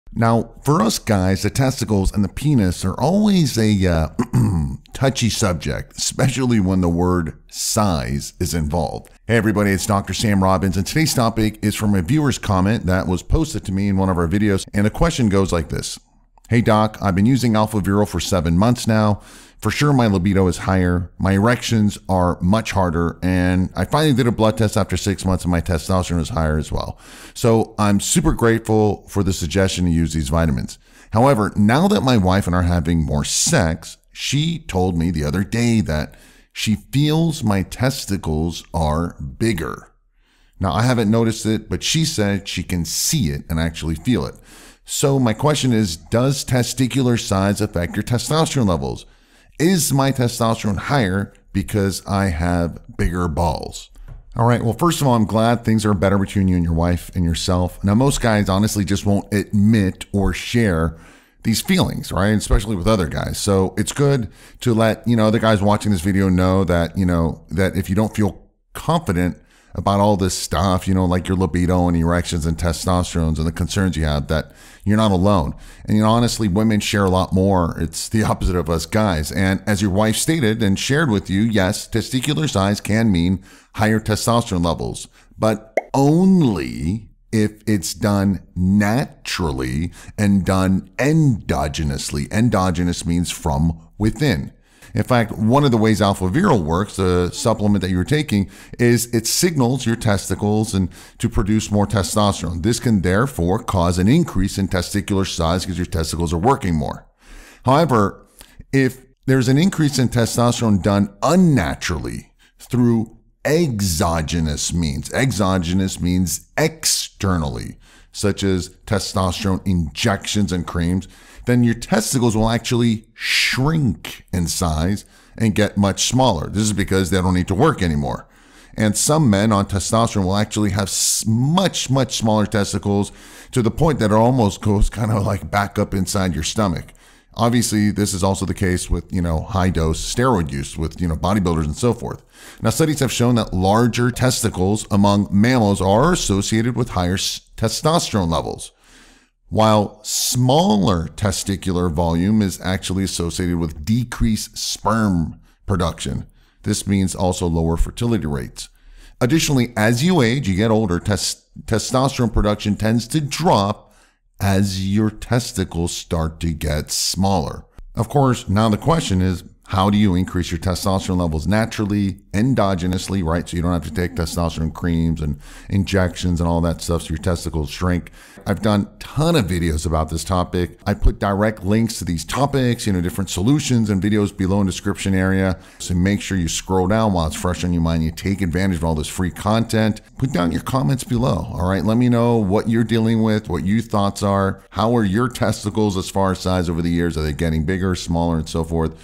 Does The Size Of Your Testicles Affect 𝐓𝐞𝐬𝐭𝐨𝐬𝐭𝐞𝐫𝐨𝐧𝐞 𝐋𝐞𝐯𝐞𝐥𝐬
Best Vitamins For 𝐈𝐧𝐜𝐫𝐞𝐚𝐬𝐢𝐧𝐠 𝐓𝐞𝐬𝐭𝐨𝐬𝐭𝐞𝐫𝐨𝐧𝐞 & Optimizing Hormones

For us guys, the testicles and penis are always a touchy subject, especially when the word “size” is involved.

And today’s topic is from a viewer’s comment that was posted to me, so make sure you post your questions below and in all my videos.

Hey Doc, I’ve been using AlphaViril for 7 months now. For sure my libido is higher. My erections are much harder. And I finally did a blood test after 6 months and my testosterone is also higher. So, I’m super grateful for the suggestion to use this vitamin.

However, now that my wife and I are having more sex, she told me the other night that she feels my testicles are bigger. I haven’t noticed it, but she said she can see it and feel it.

So does testicular size affect your testosterone levels? Is my testosterone higher because I have bigger balls?

I’m glad things are better between your wife and yourself. Most guys just won’t admit or share these feelings with other guys. So, it’s good to let other guys know if they don’t feel confident about all of the libido and erection and testosterone concerns you have.

Women share a lot more. It’s the opposite of us guys.

And, as your wife stated and shared with you, yes - testicular size can mean higher testosterone levels, but ONLY if it’s done naturally and done endogenously - that means, from WITHIN.

And I’ll show you how in a minute.

In fact, ONE of the ways AlphaViril works is by signaling to your testicles to produce more testosterone. This can then cause an increase in testicular size.

However, if it’s done unnaturally, through exogenous means, which means EXTERNALLY - such as testosterone injections and creams, then the testicles will actually SHRINK in size and get much smaller.

Some men on testosterone will actually have such small testicles that it almost goes back up inside your stomach. Obviously, this is also the case with high dose steroid use

Studies have shown that larger testicles among mammals are associated with higher testosterone levels, while smaller testicular volume is associated with decreased sperm production. This means a lower fertility rate.

Additionally, as you age, testosterone production tends to drop as your testicles start to get smaller.

So, there you have it, generally speaking, larger teticles will indicate higher testosterone levels.

And I’ve done a ton of videos about this topic and how you can naturally and quickly increase your testosterone levels.

Thanks
DrSamRobbins